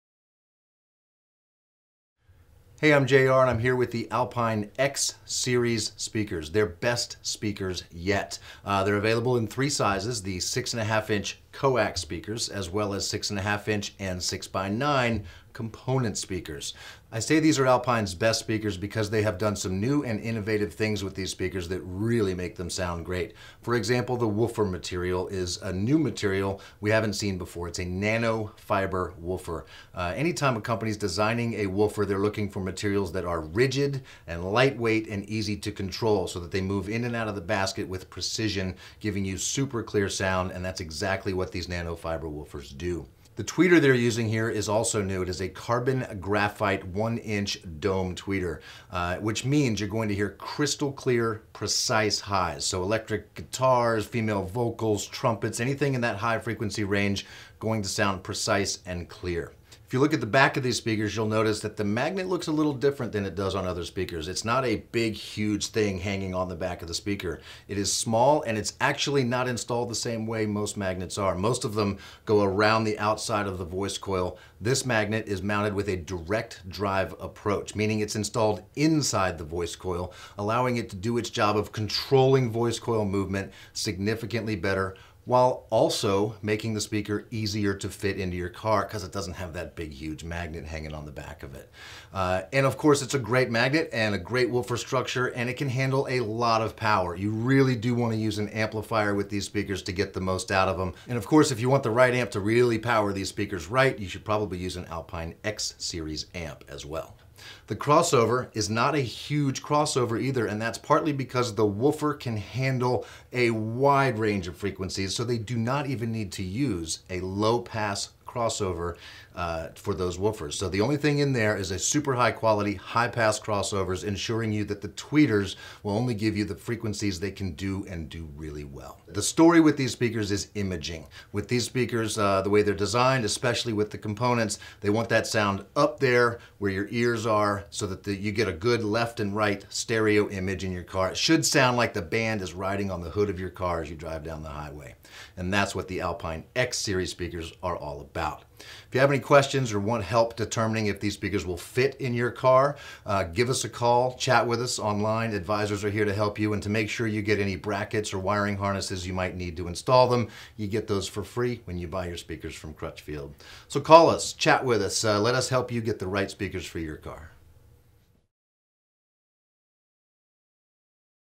Alpine X-Series car speakers | Crutchfield video
Alpine's X-Series speakers are their top-of-the line models, delivering beautiful sound when powered by an amp. They're available in 2-way coaxial and as a component system with separate tweeters.